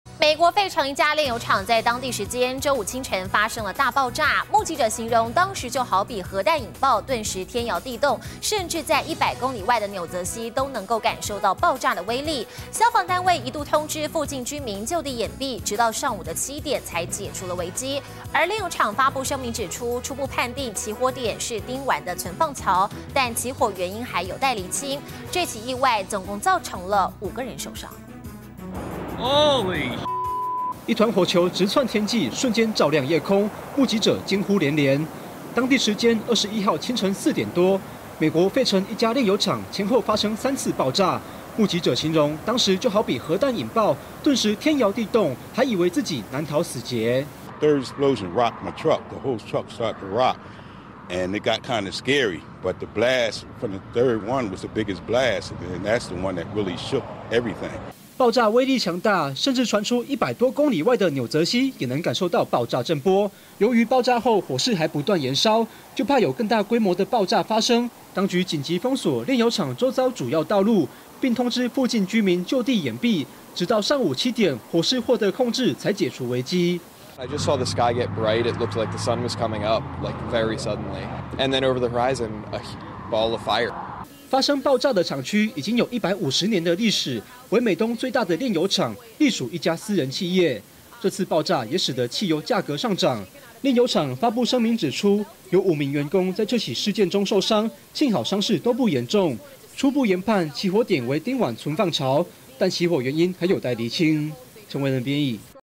震波傳百公里外紐澤西 當局緊急封鎖周遭道路
美國費城一家煉油廠在當地時間週五清晨發生爆炸，目擊者形容，當時就好比核彈引爆，頓時天搖地動，甚至一百公里外的紐澤西都能感受到爆炸威力。消防單位一度通知附近居民就地掩蔽，直到上午七點才解除危機。煉油廠發布聲明指出，初步判定起火點為丁烷的存放槽，但起火原因還有待釐清，這起意外共造成5人受傷。